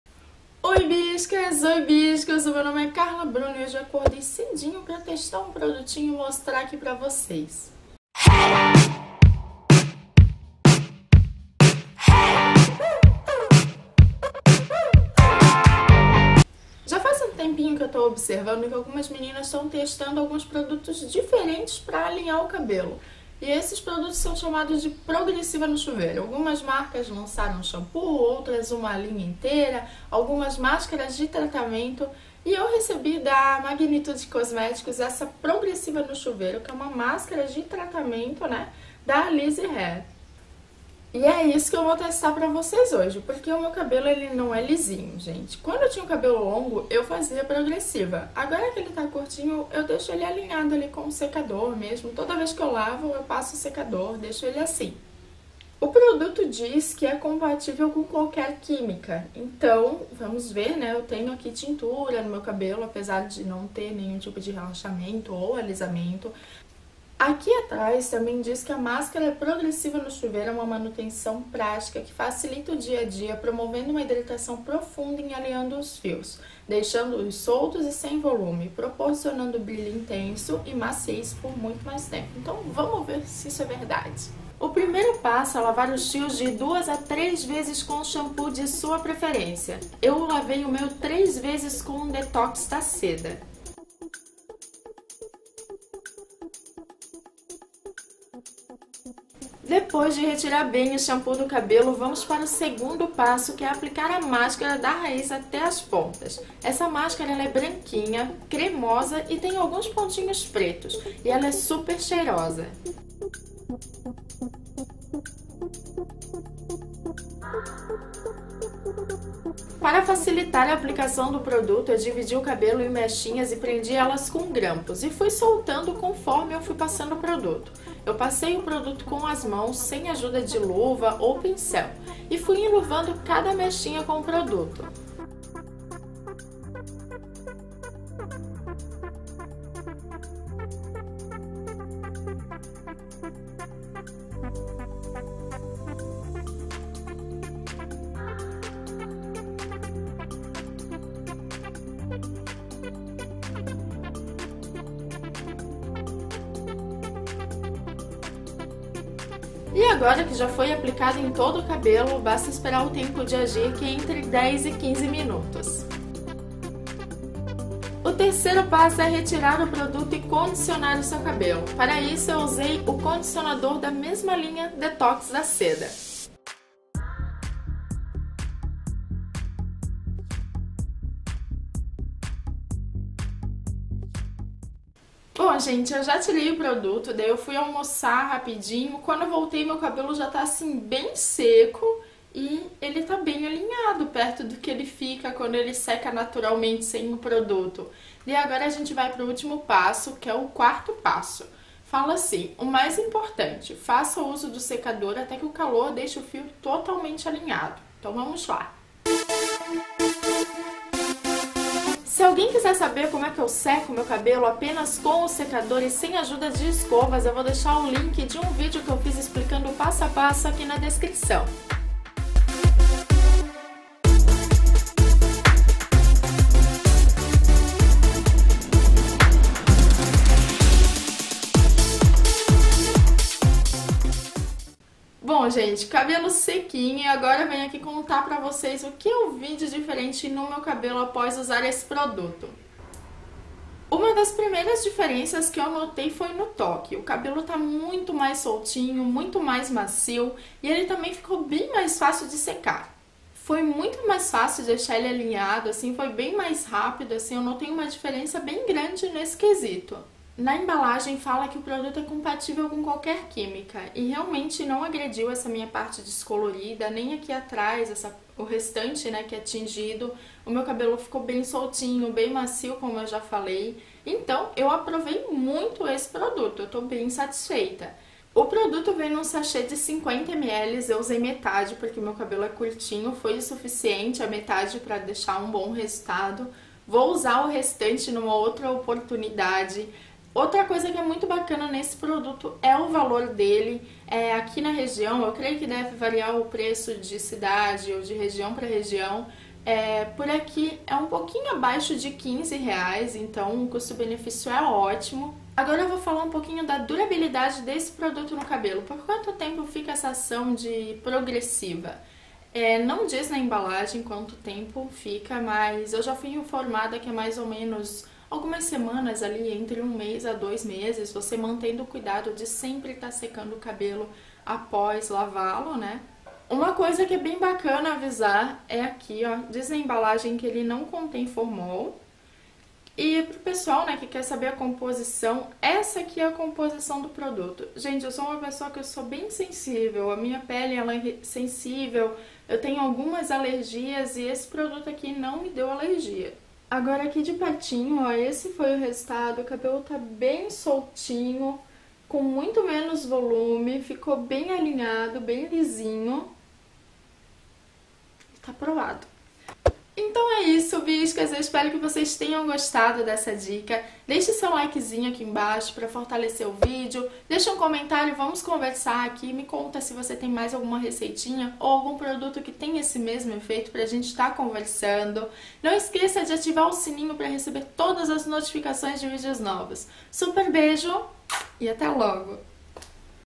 Testei - Progressiva no Chuveiro Alise Hair
Oii Biscas, Ooi Biscos!
ESSE VÍDEO NÃO É 'PUBLIPOST'
Confiram o passo a passo da aplicação e também uma resenha sincera sobre o resultado!